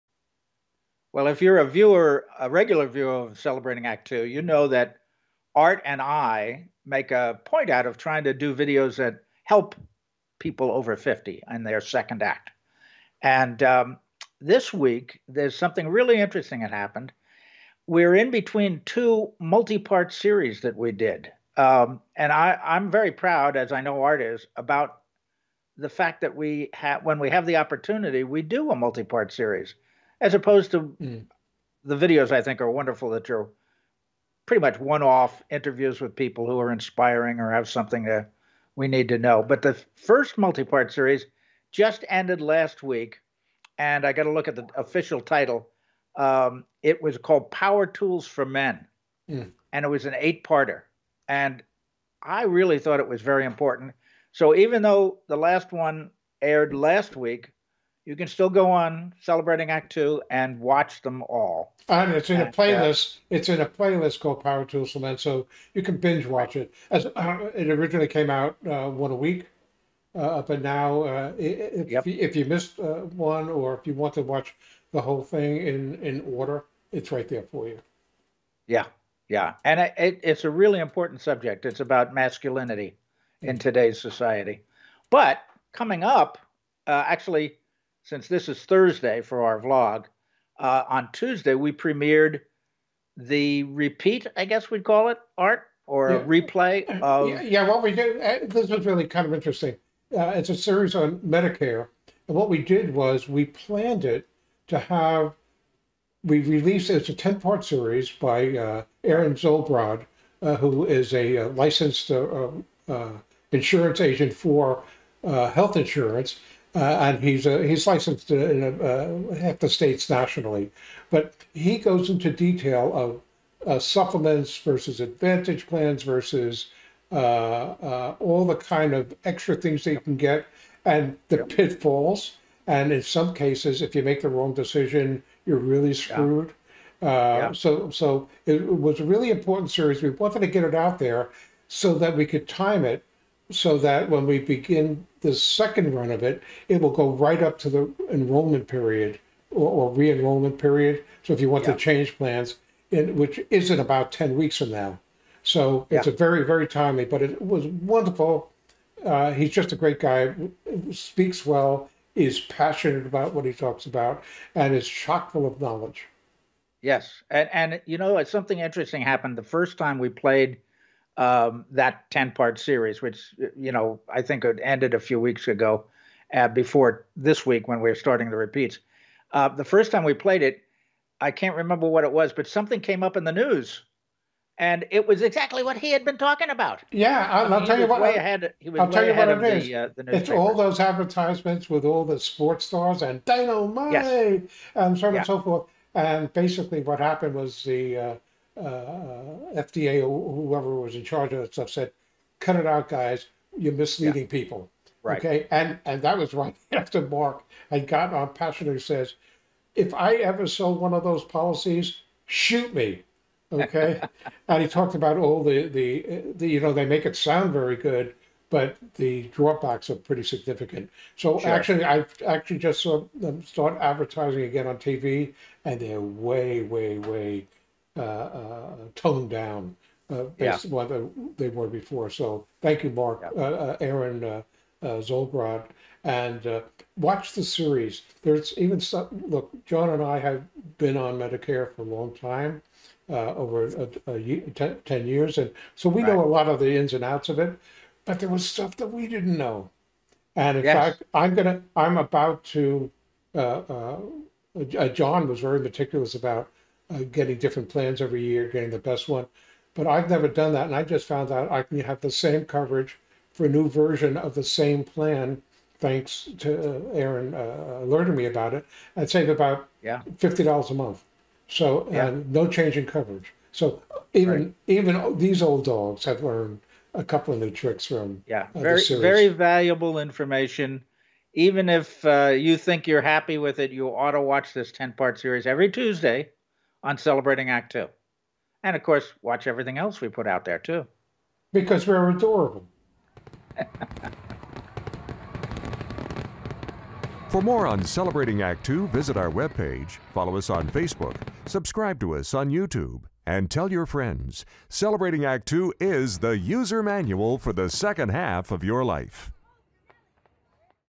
Open Enrollment Coming Soon
PREPARE  This exclusive ten part series by licensed insurance agent Aaron Zolbrod will give you information to help make intelligent choices when the annual open enrollment period opens October 15 to Dec 7, 2023